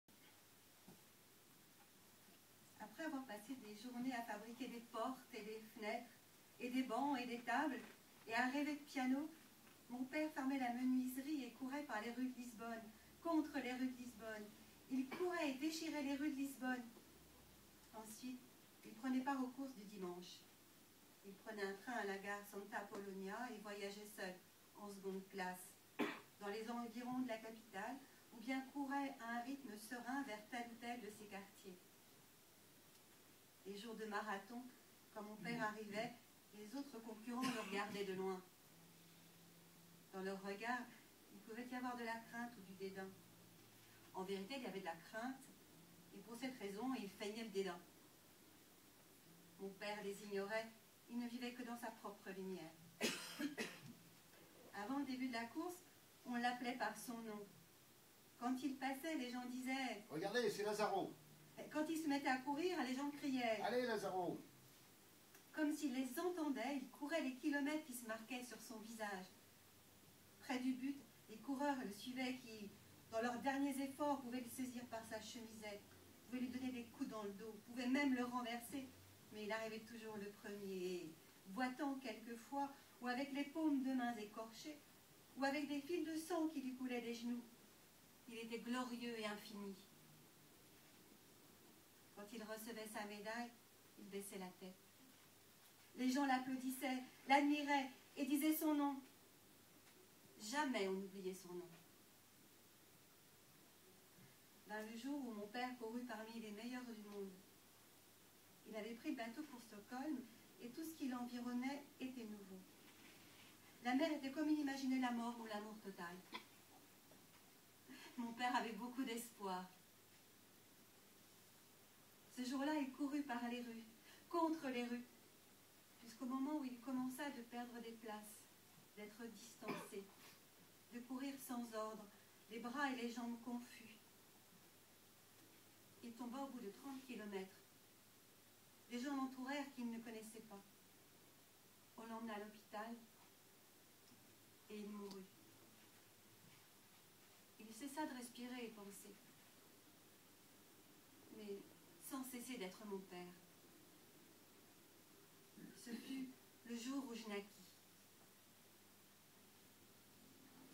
Lecture de "Le Cimetière des Pianos", de José Luís Peixoto, à Guérande
Lecture d'un extrait de "Le Cimetière des Pianos, de José Luís Peixoto.
Lecture par Pascale et Olivier à la Médiathèque de Gérande.
7 Mars 2012